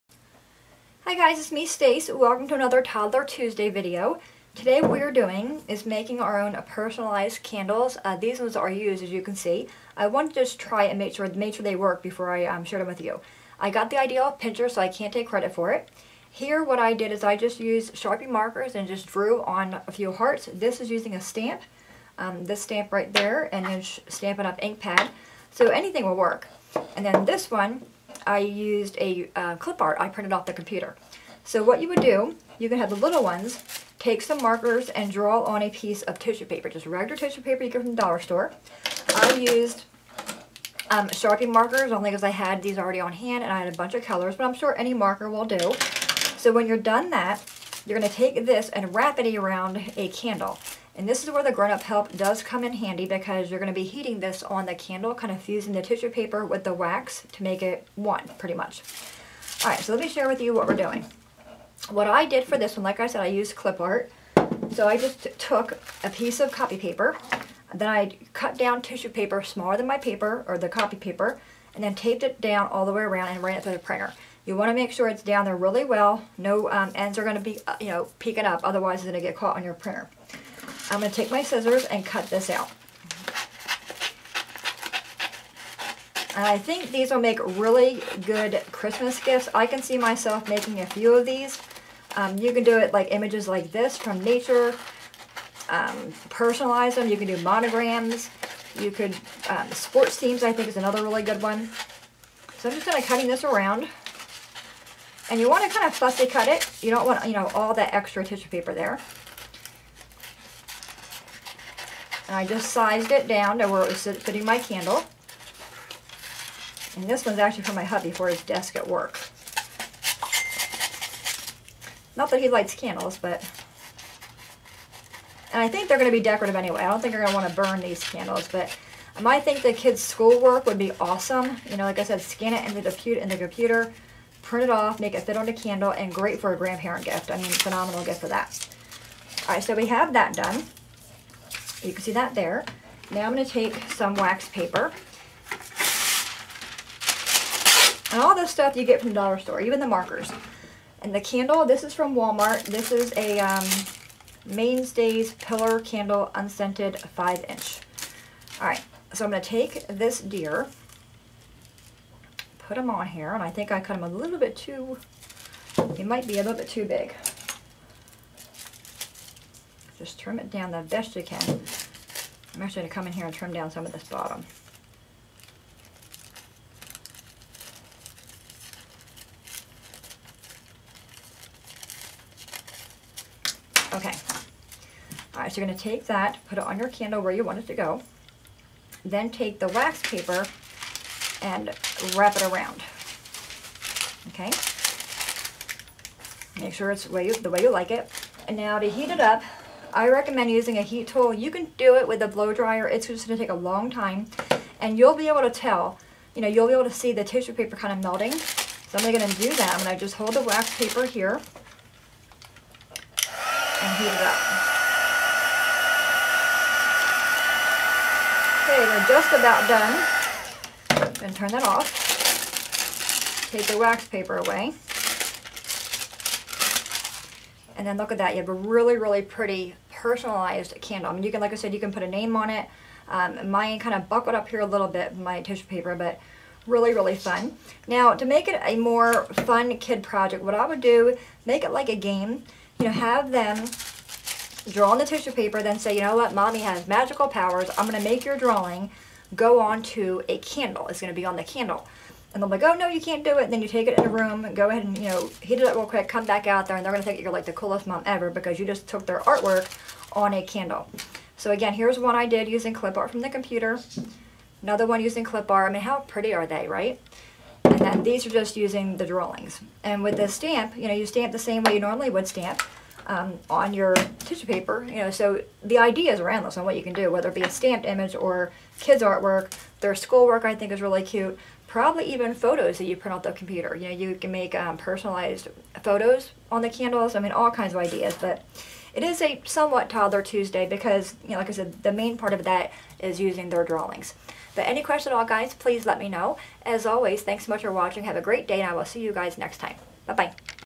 Personalized Candles
Hi everyone,
This is somewhat of a Toddler Tuesday, it just requires a little bit of a grownup's help!
Hope you enjoy, Hugs ~ Stacy